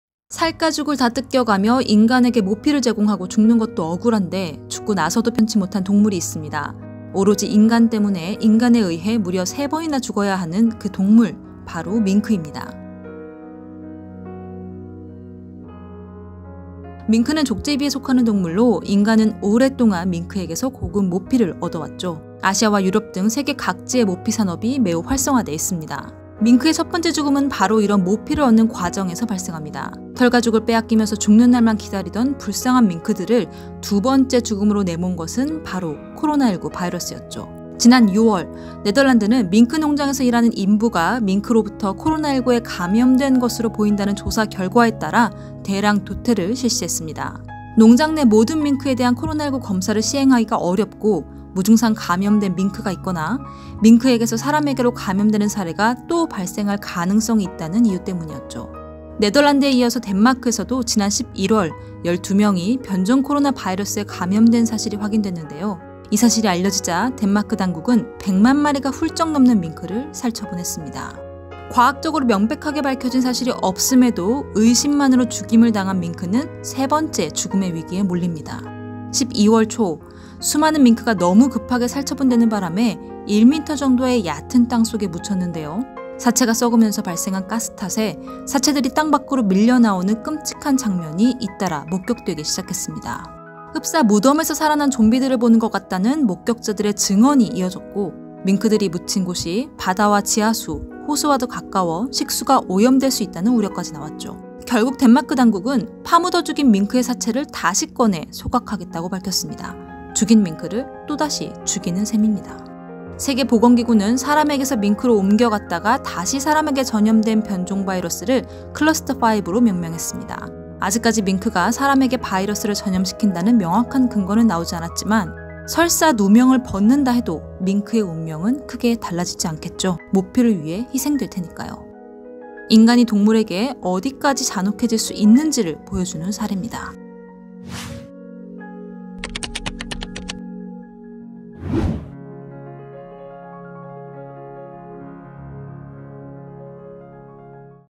변종코로나 탓에 '좀비'가 된 밍크의 슬픈 사연 들어주세요!!
동물이 무슨 잘못?! 인간 때문에 피해를 본 밍크의 억울한 사연, [이슈플릭스]에서 꼭 확인해주세요!!!